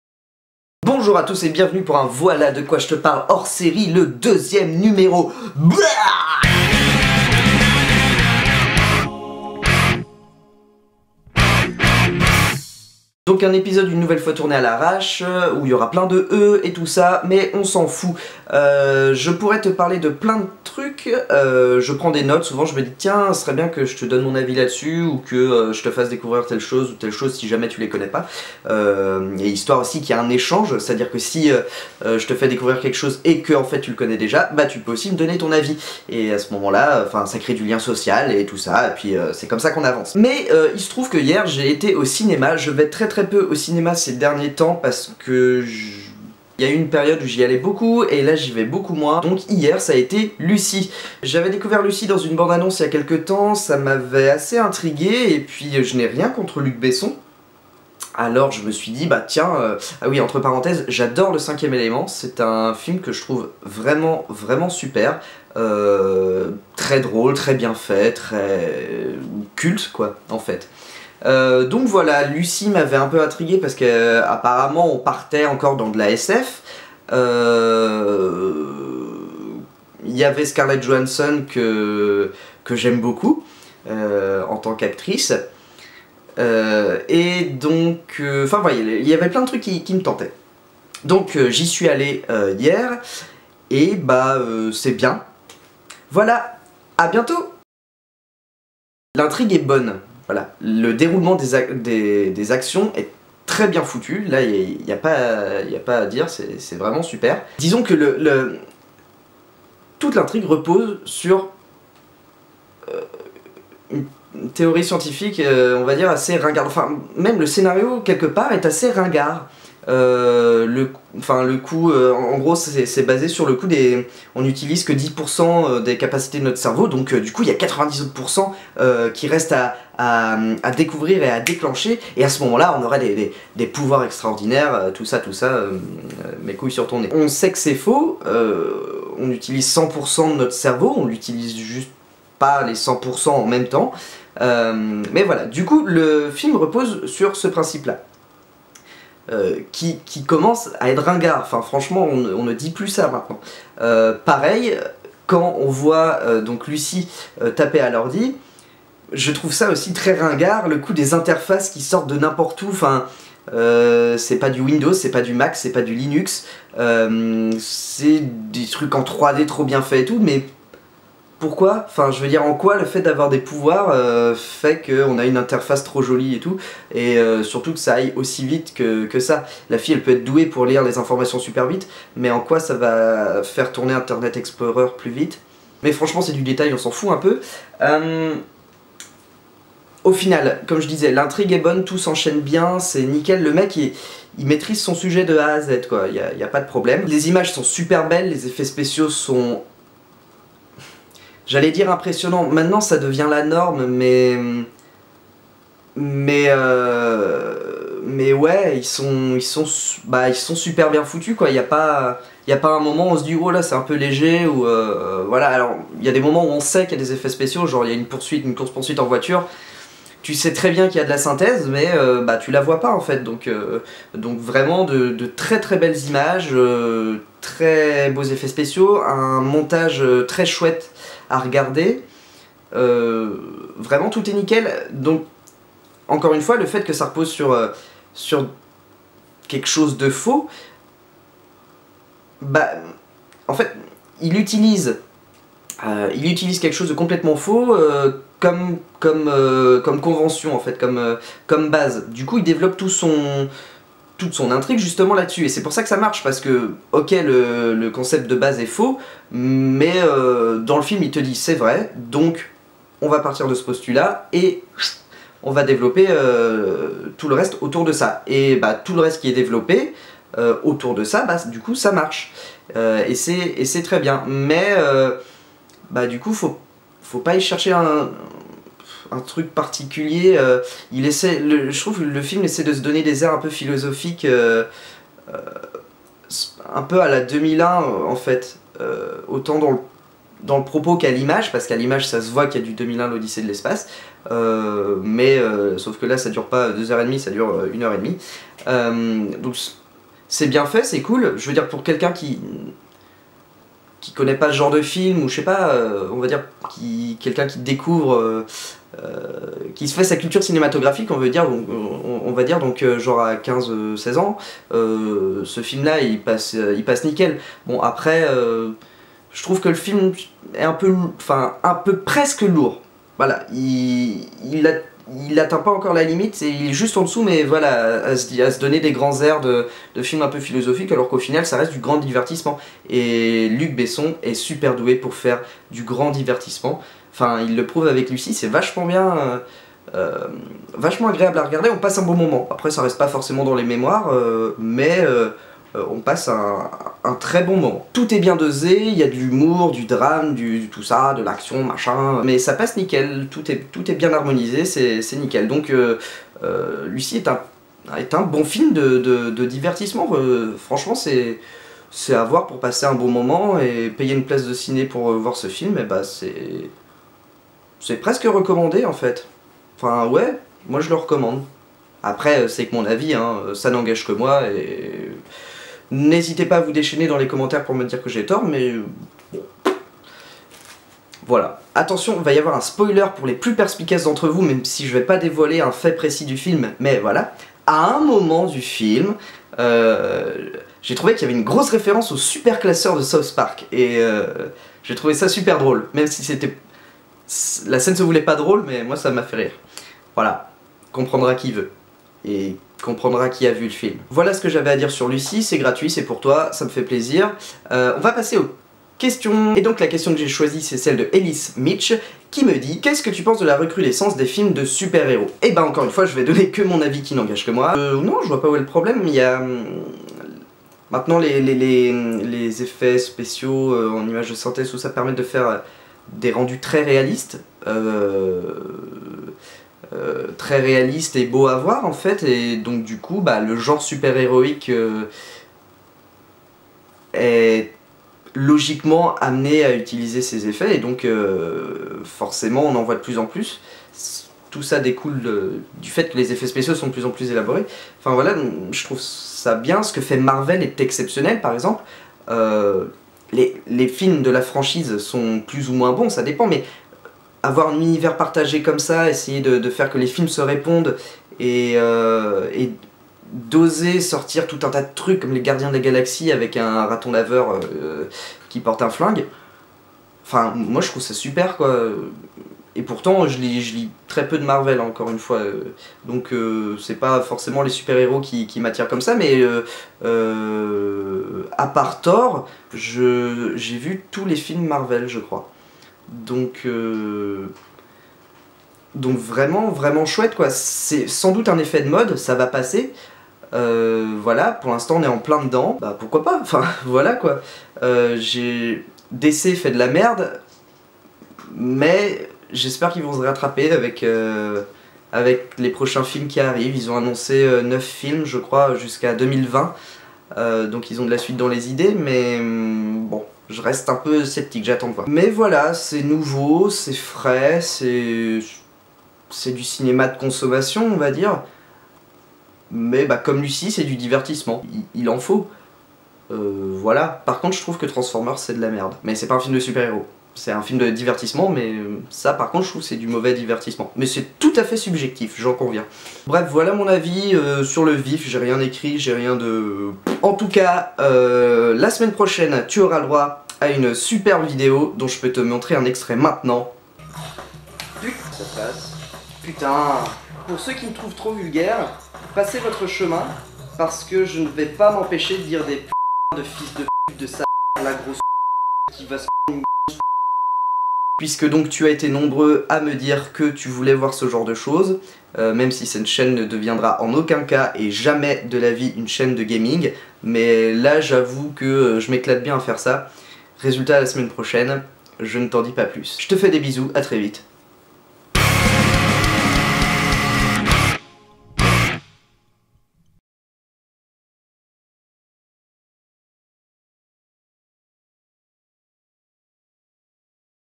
Voilà de quoi j'te parle HS02 - Lucy & films de Super héros
Deuxième Hors-série de ma chronique. Hors-série parce que non écrit à l'avance.

Cette fois, j'évoque le film Lucy de Luc Besson ainsi que les films de super-héros d'une manière générale.